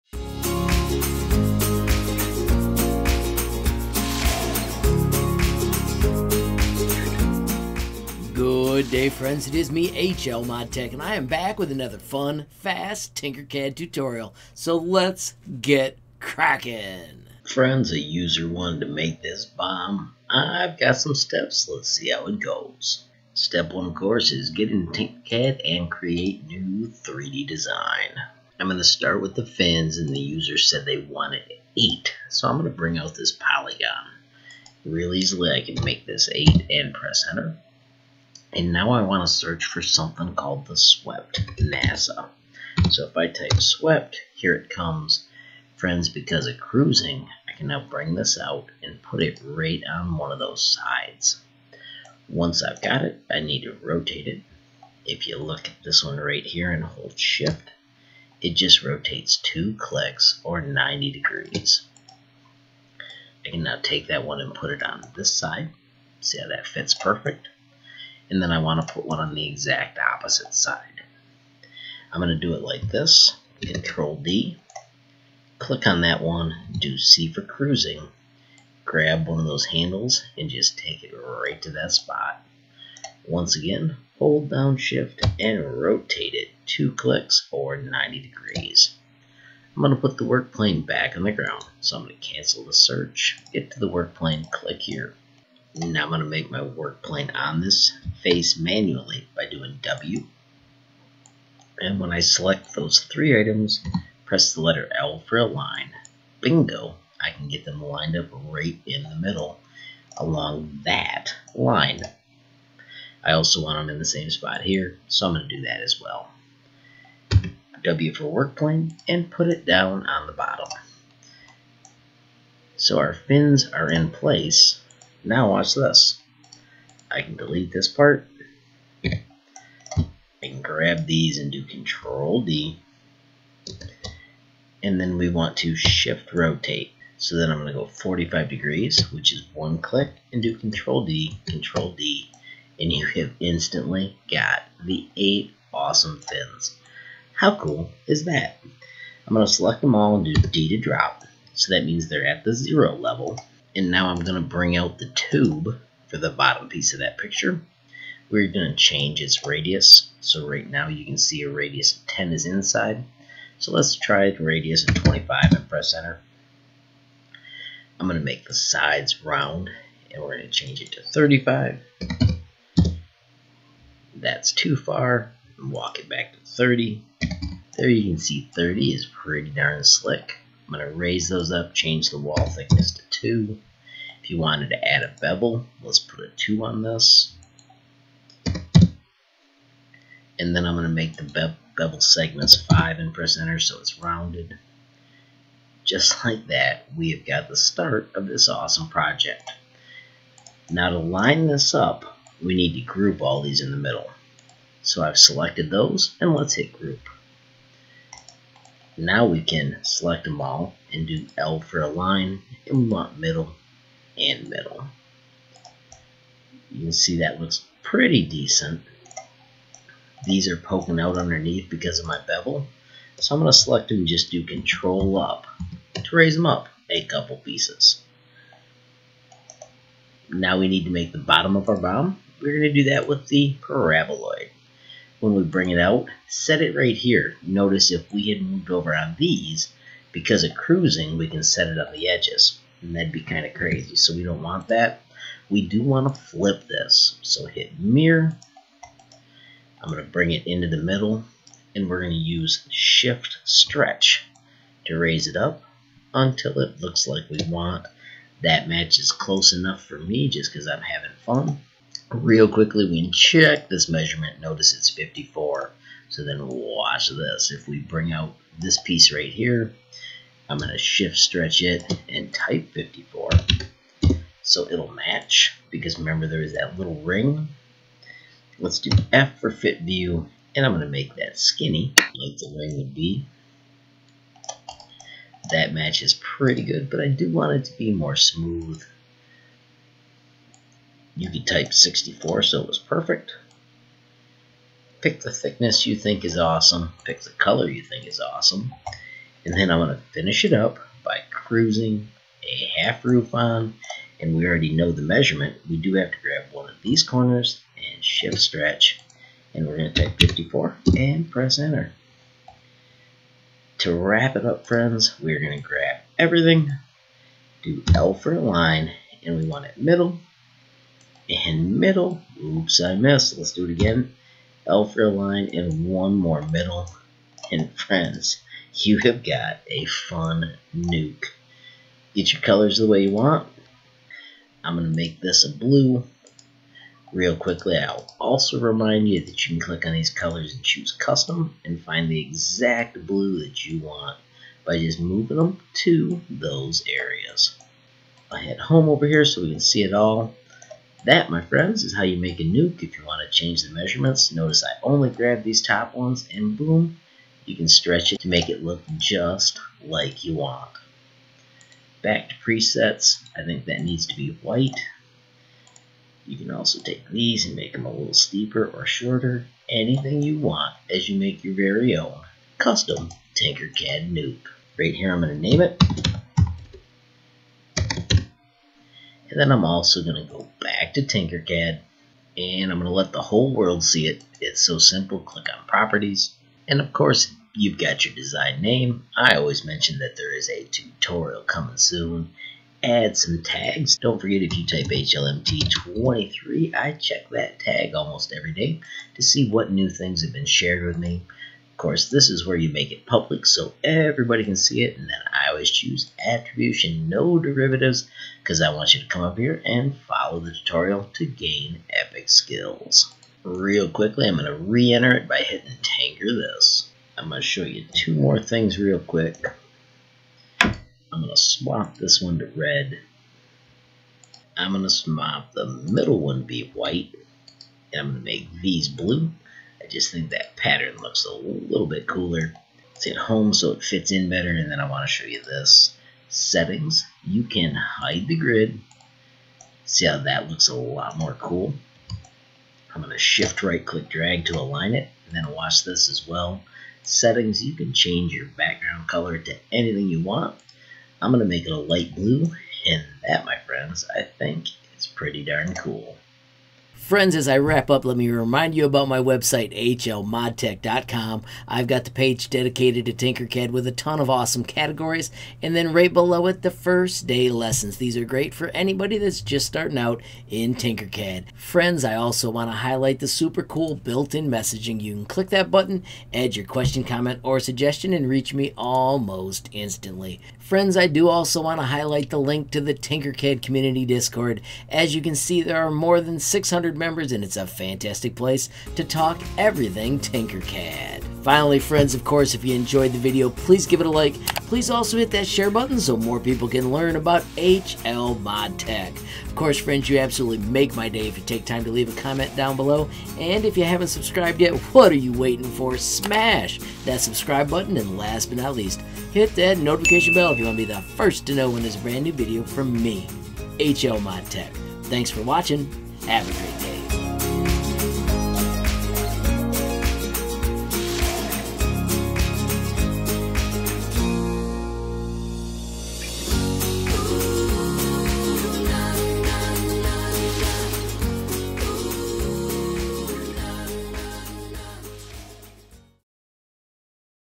Absolute Beginner Tinkercad Tutorial to Make a Cartoon Nuke
Have a glorious day and keep Tinkering! This Absolute Beginner Tinkercad  to Make a Cartoon Nuke is fun fast and full of useful skills. This Nuke is just for fun. The new cruising feature, many basic skills, and all the shortcuts are in there. Have fun with your car and please share if you are proud of it.

Make my day by giving the Tinkercad project a reaction too.

Make something incredible yourself? Use the share techniques in the video and I may also showcase your skills.